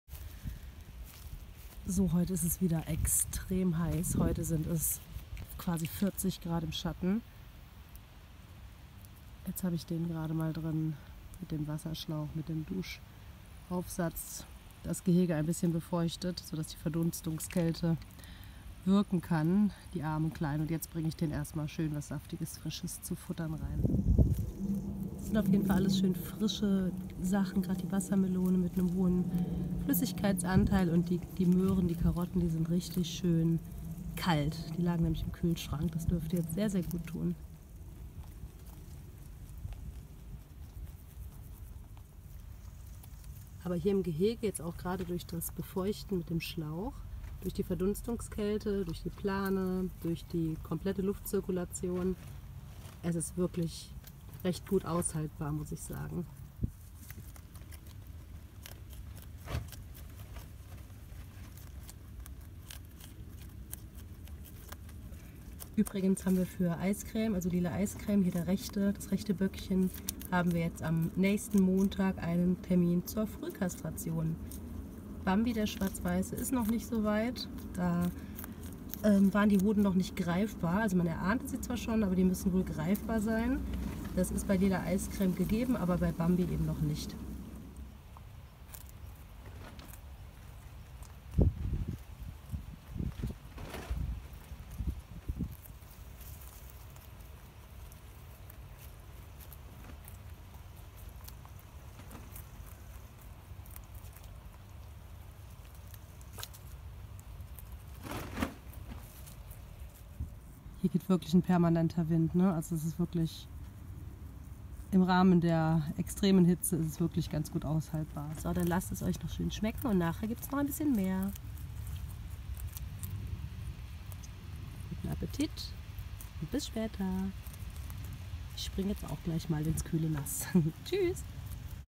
Kaninchen: Tipps gegen Hitze und Info Frühkastration
Knapp 40 Grad im Schatten.. 🙈